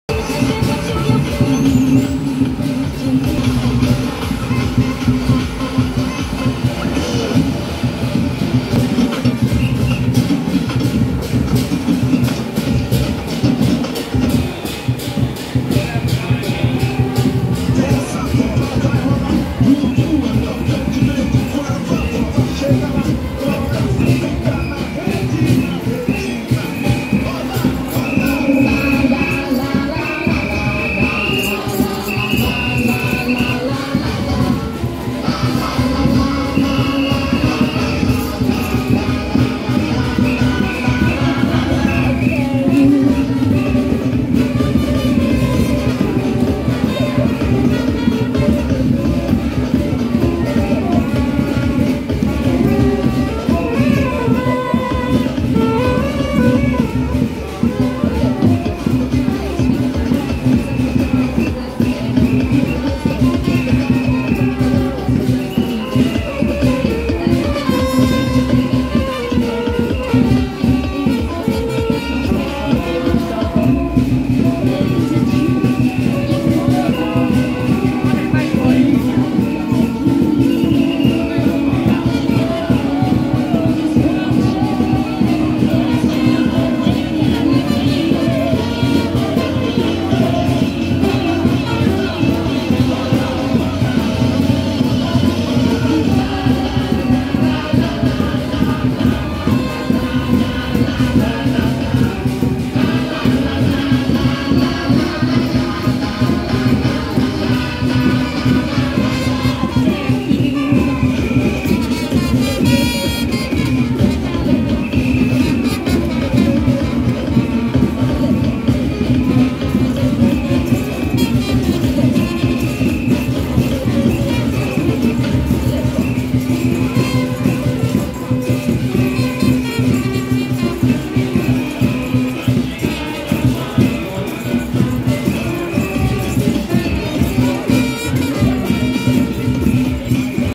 【台中鳳聲女子樂團】~111台北葫蘆寺建廟75週年恭迎眾神水陸除瘟巡安賜福遶境
台中鳳聲女子樂團